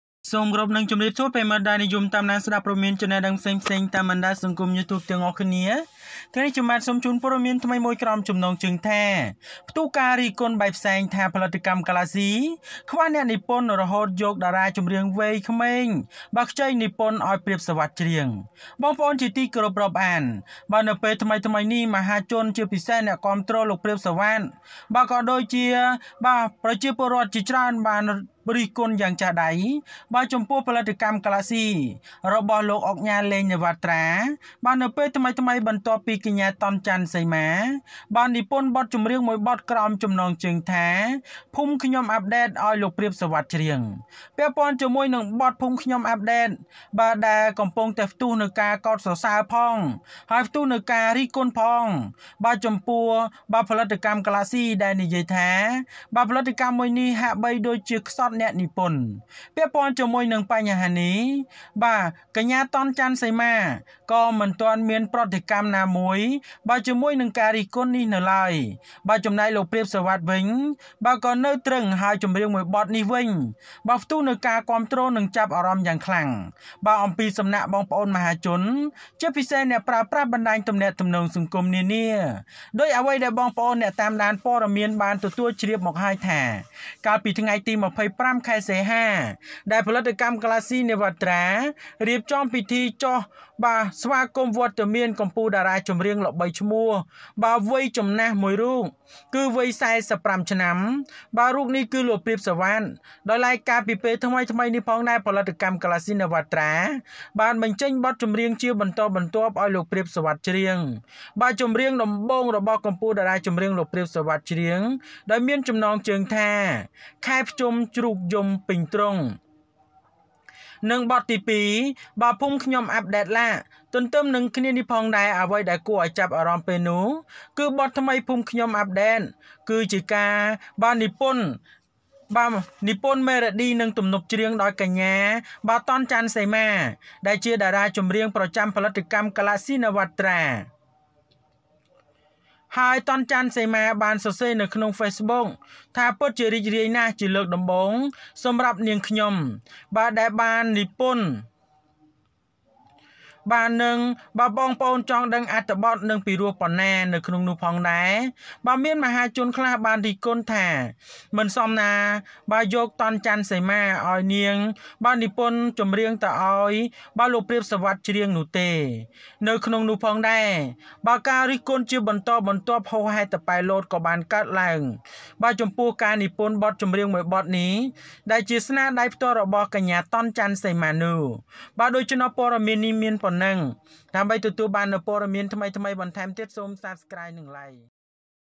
ផ្ទុះការរិះគន់បែកផ្សែង! Galaxy ខ្វះអ្នកនិពន្ធ រហូតយកតារាចម្រៀងក្មេងខ្ចី និពន្ធឲ្យព្រាប សុវត្ថិច្រៀង
ផ្ទុះការរិះគន់បែកផ្សែង! Galaxy ខ្វះអ្នកនិពន្ធ រហូតយកតារាចម្រៀងក្មេងខ្ចី និពន្ធឲ្យព្រាប សុវត្ថិច្រៀង Explosion of criticism!  Galaxy lacks writers until young singer writes for Preap Sovath to sing.